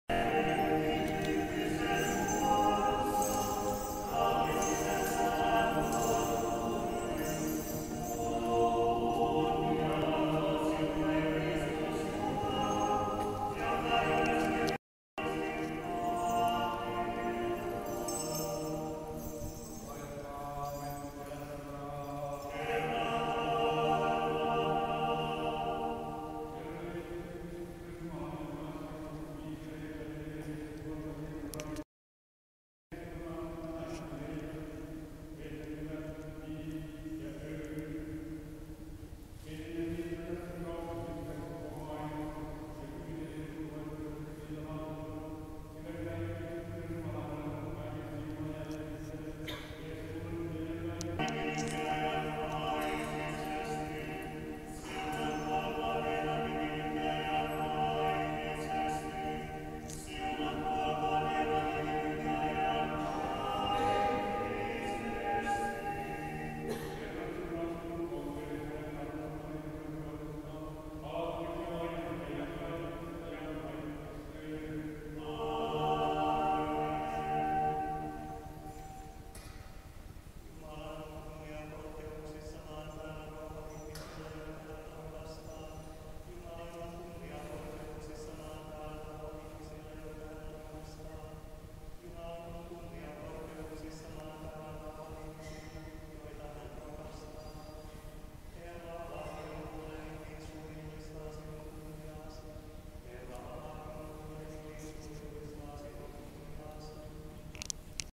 Helsinki - in the orthodox Uspenski church
Evening service before Epiphany